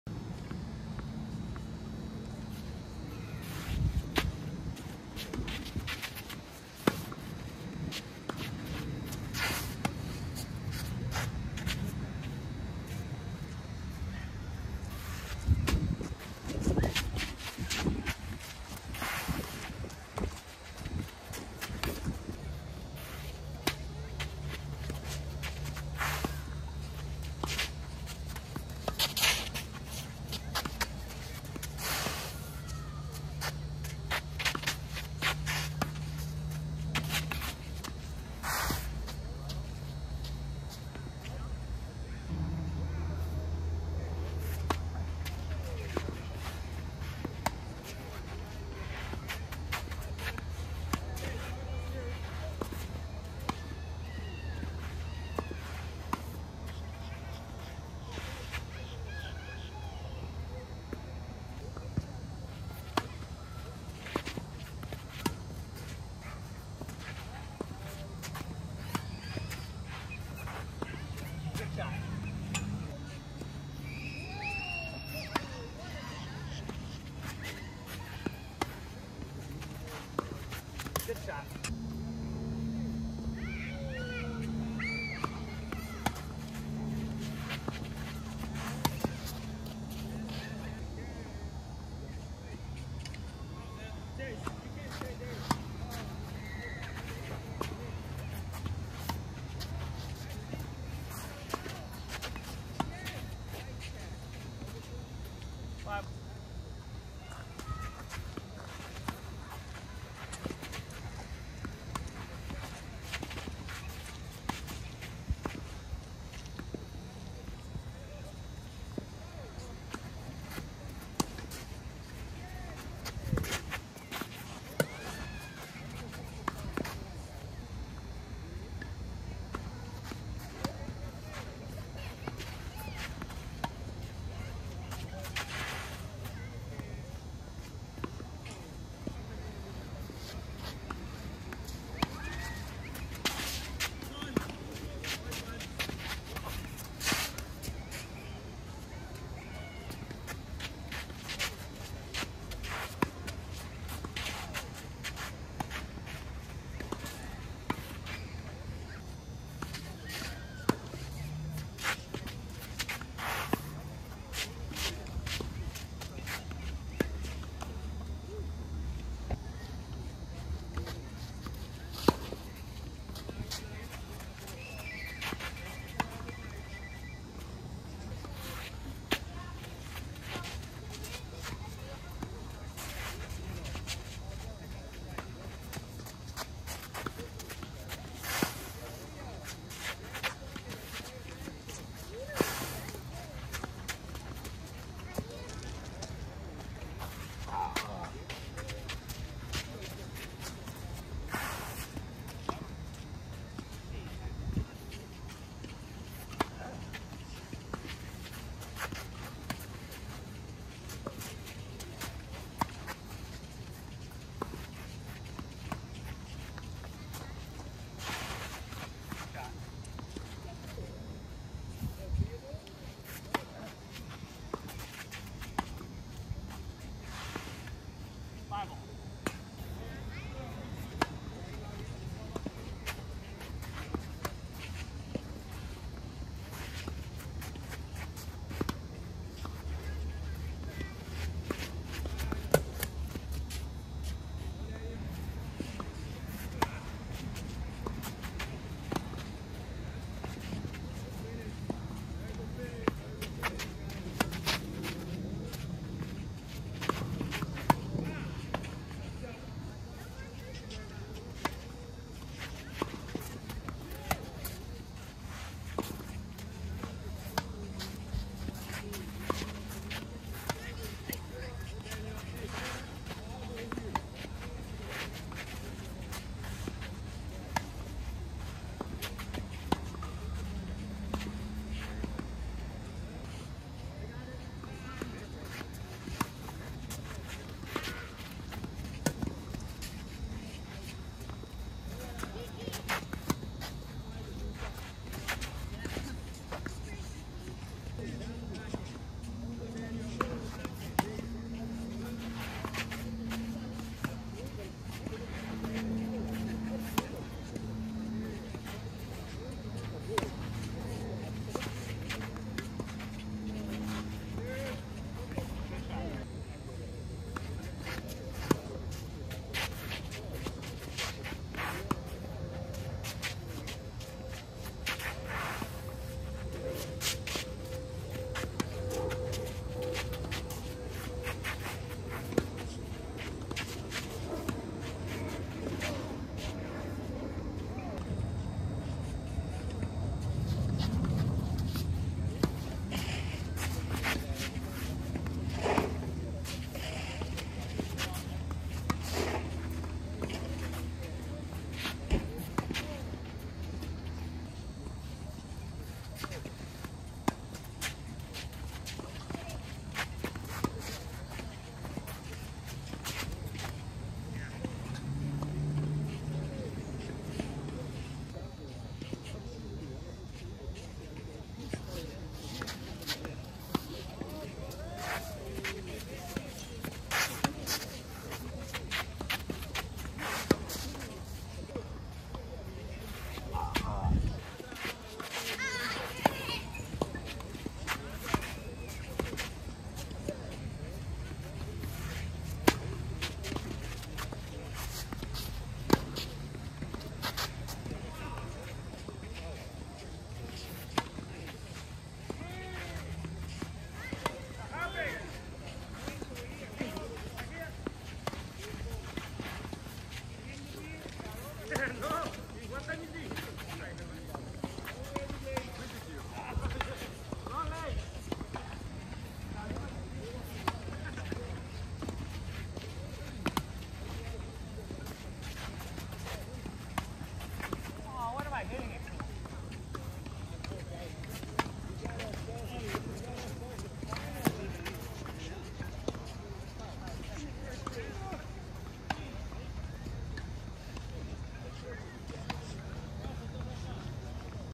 USTA Rated 4.0 Men's Singles Match - Baseliner vs The Ultimate Slicer Clay Court Match
This is a USTA Rated 4.0 Singles match I played recently. This match is played on a green clay court. I won this set 6-4.
Me = Black
Opponent = Gray

Gear I used to film this video:

The racket I use